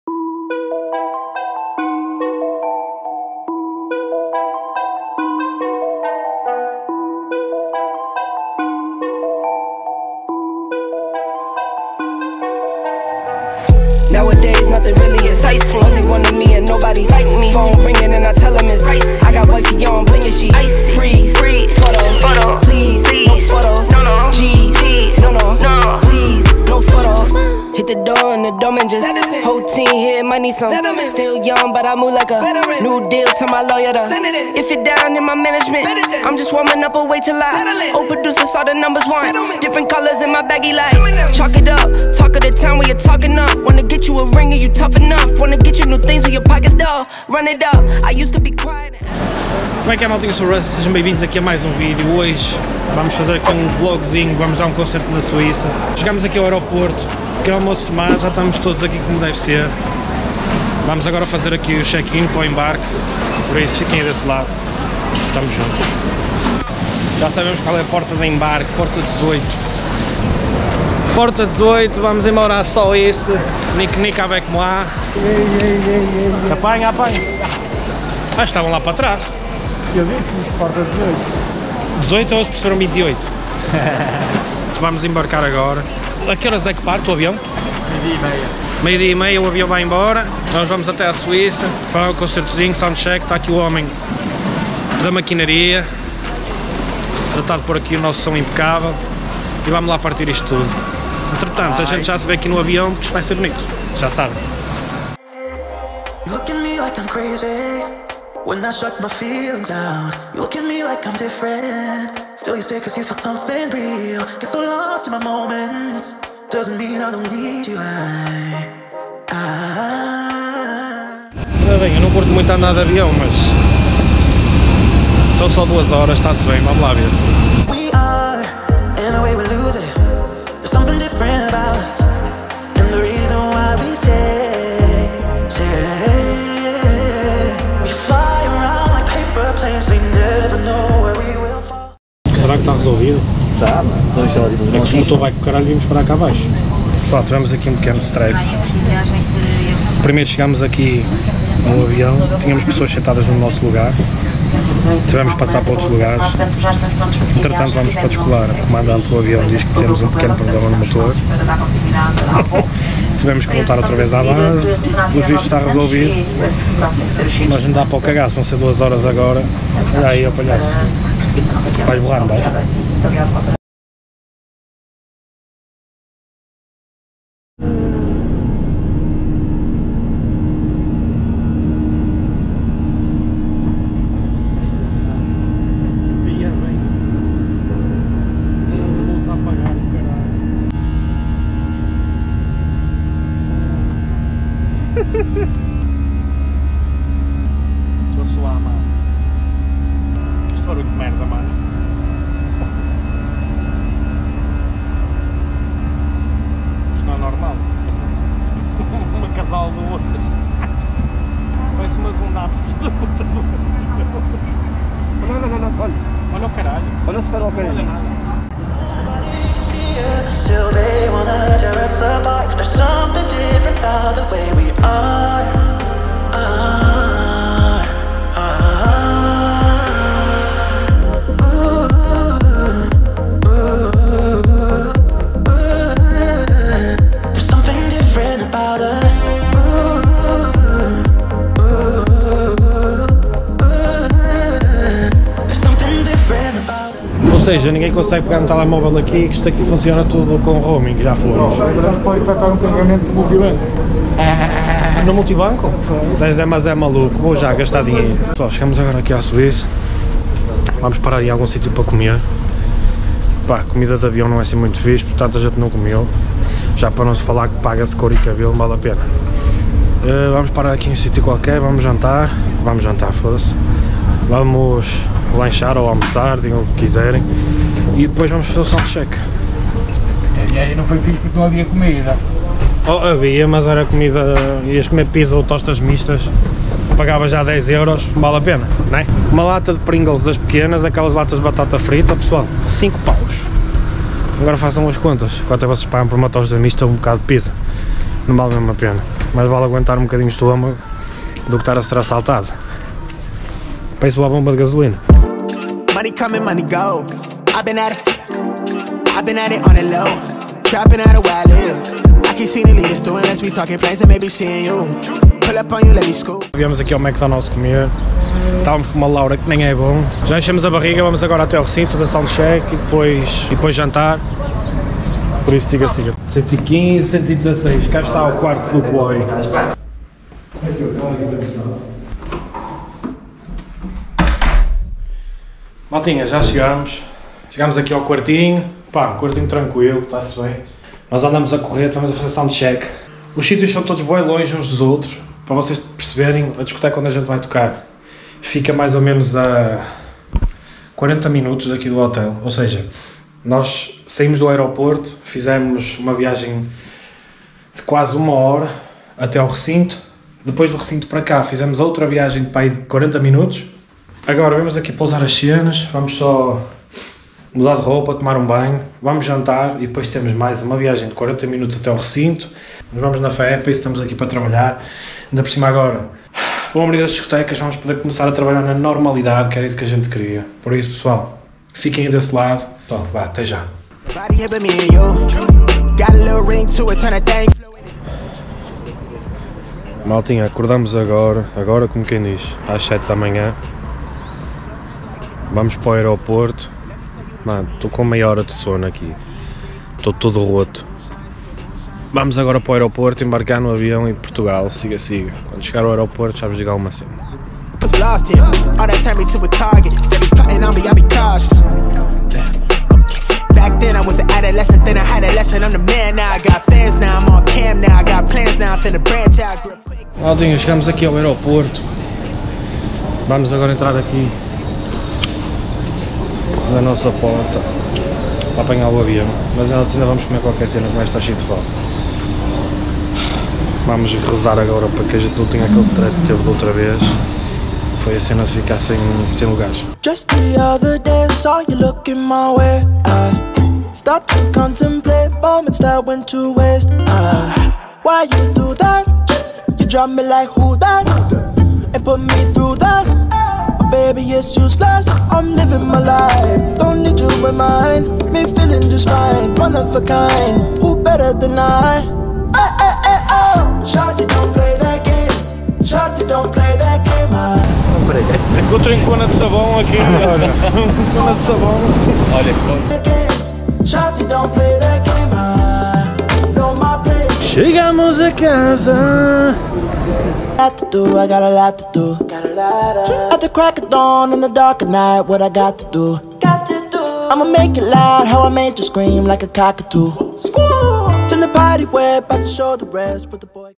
DEMOS UM SHOW NA SUÍÇA !! (Borrei-me todo no avião 😰) ✈️ ‹ Rustymusik ›
Mekie família? fica aqui um pequeno vlog daquela que foi a nossa viagem à Suíça. O voo para lá não correu assim tão bem pelo facto de ter havido um erro com os bilhetes. Haviam pessoas que tinham  o mesmo numero que nós obrigando-nos a ir sentados noutros lugares.
Como se não bastasse, o comandante do avião decide avisar antes da partida que tinha havido um pequeno problema no arranque de um dos motores do avião.
Estivemos à espera que a situação fosse resolvida e como é obvio, ficamos ainda mais borrados por causa da turbulência que se fez sentir após a descolagem.

Espero que curtam o video.. Siga filhoooo!

♛ RUSTY ♛
★ Dj/Produtor Oficial do Piruka
★ Swag On

Tamos juntos família 🖤